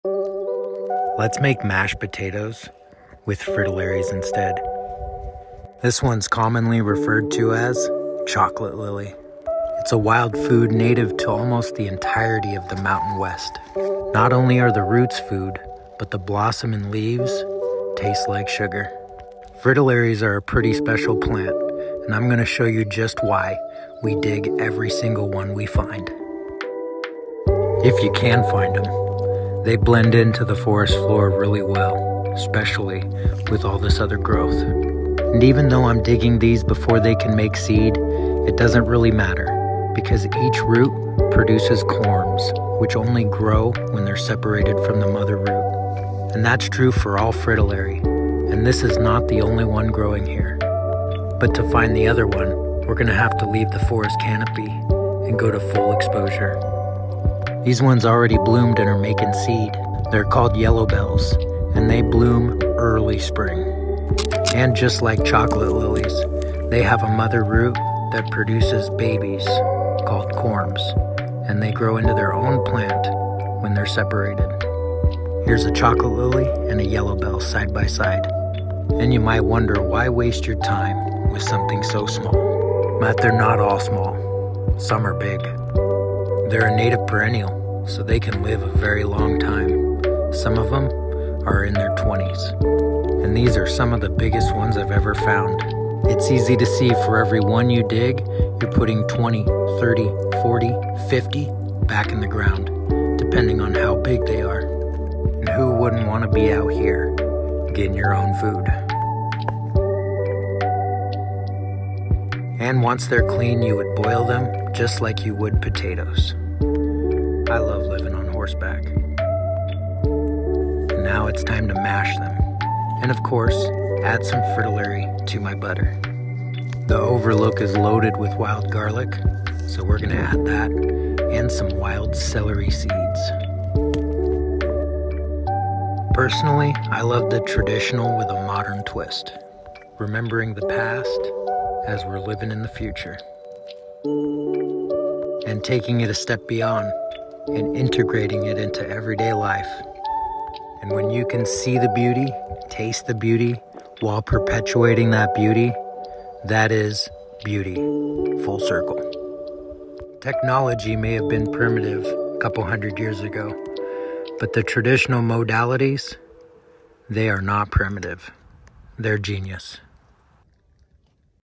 BETTER than mashed potatoes!
Making mashed potatoes with wild fritillary! Fritilaria Atropurpurea and fritilaria Pudica taste better than they sound. Much of the native wild foods thrive with harvest and fritillary are a great example of that. They are a global genus found on every continent not covered in ice. Some species are protected and it is illegal to dig them, however.. that protection is unnecessary and only contributes to their diminished presence because every fritillary in the world propagates with corms aswell as seed. Traditional foods may be small but their food value is on a whole other planet compyto the watered down domestic vegetables.